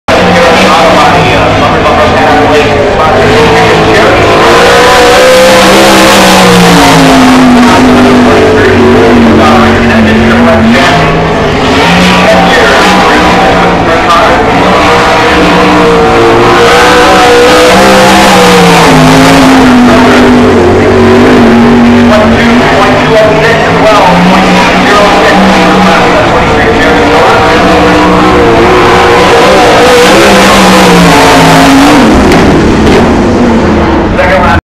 Jereme Schroeder Qualifying Laps
Jereme Schroeder takes his qualifying laps in the # 43 machine duriing the IRA Outlaw Sprint show at Plymouth, WI July 25th, 2011